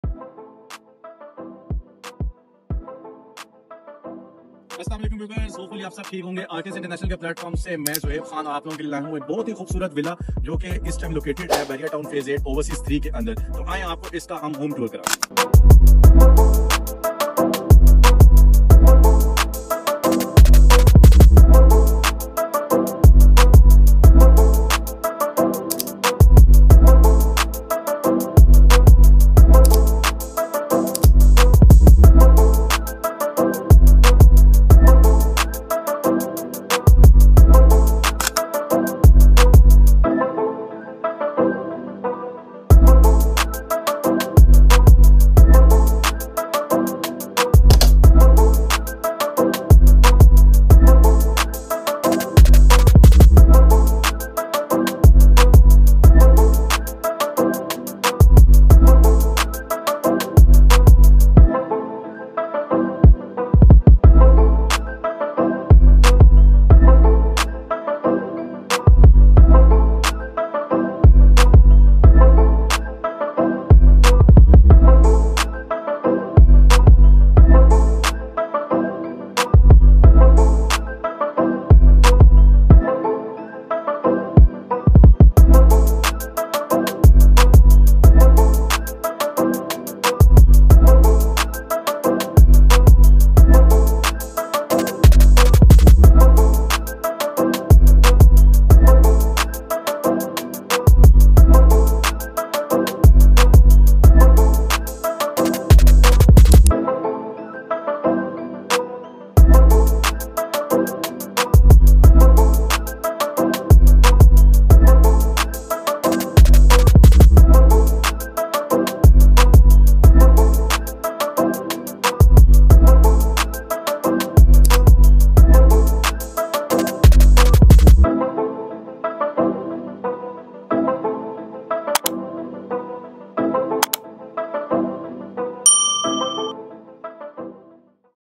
926 Villa for Sale Bahria Town Phase 8 Overseas 3| Villa in Bahria Town for Sale
Discover the luxury of a 926 Villa for sale in Bahria Town Phase 8 Overseas 3!

Explore premium villa options in Bahria Town and find your dream home.